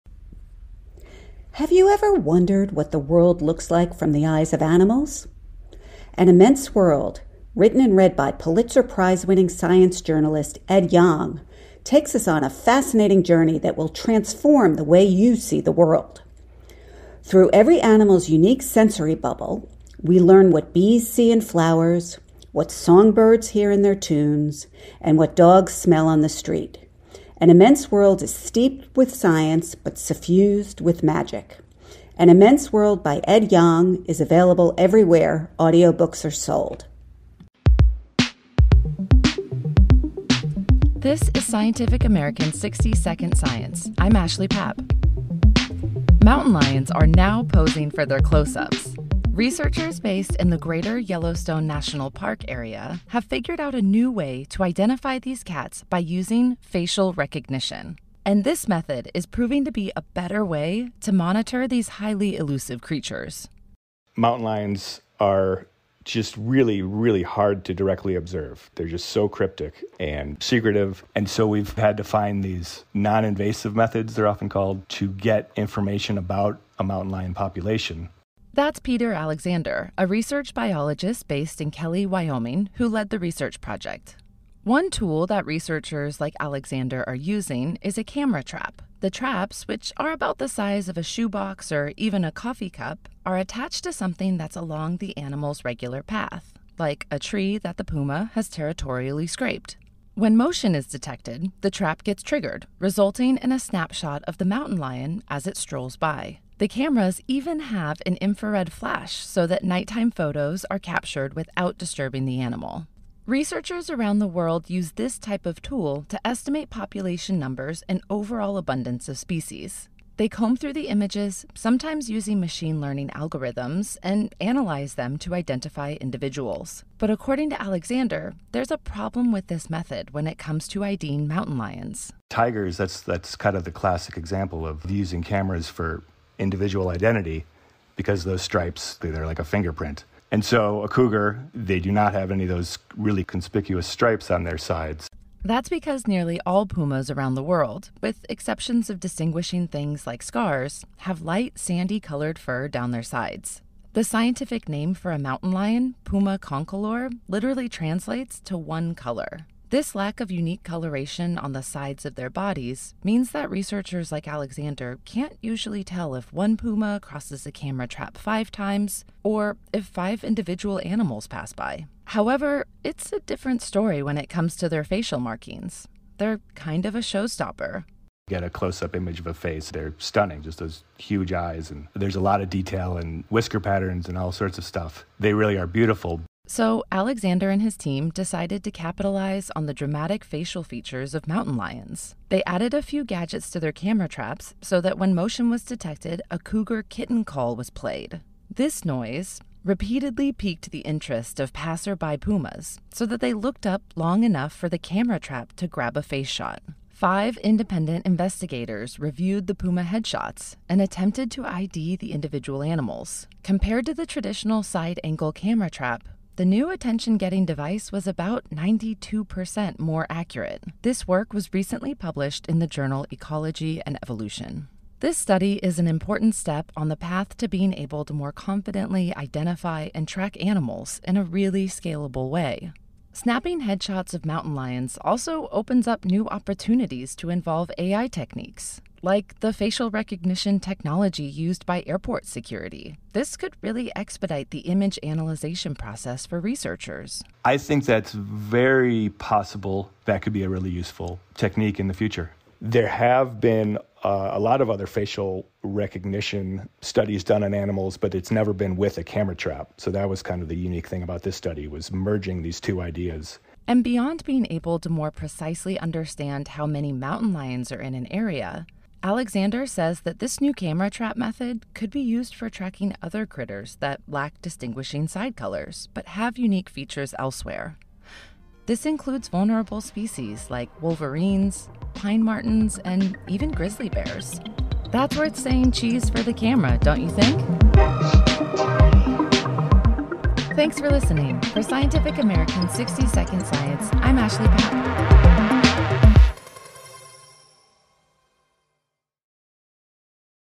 How AI Facial Recognition is Helping Conserve Pumas | 60-Second Science
Researchers tricked out conventional camera traps to snap headshots of Puma concolor, revealing a better way to track the elusive species.
/9816352